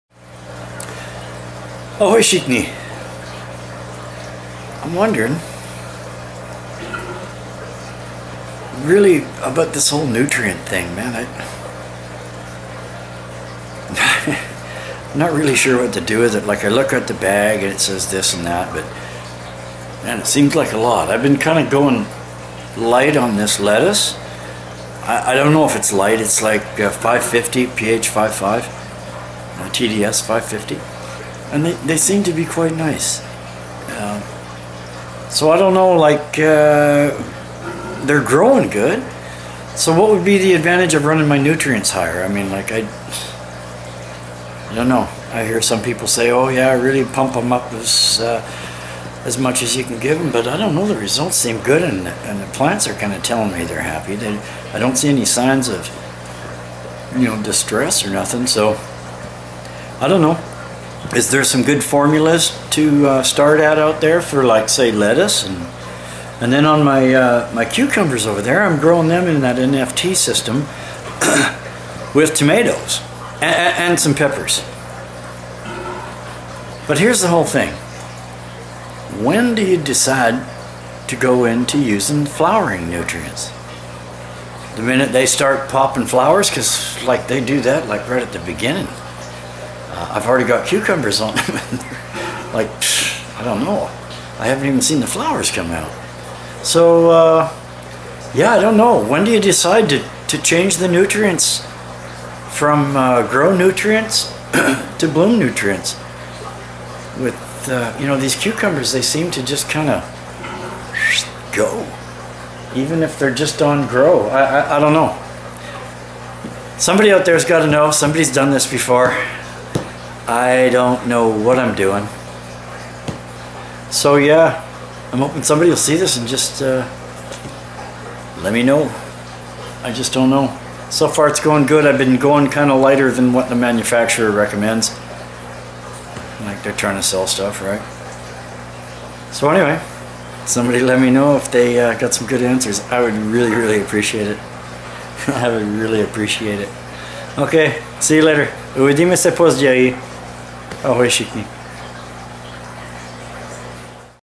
Hydroponic Greenhouse, Nutrient question.mov
This is a project I started to see if it was at all possible to supplement a family's vegetable needs. By seeing what I can do in my limited area I thought it may give me a better idea of how much room it would require to feed a family of four.if nothing else I want to be productive in a way that is beneficial to my family,my self my neighbours and my over all community. It's my opinion that we all need to move away from the model of central distribution which makes us far to vulnerable if it goes to shit. It's my opinion we need to be increasingly more reliant on our selves for our own well being instead of settling for the bullshit that's foisted upon us by the main stream. This is one of my attempts.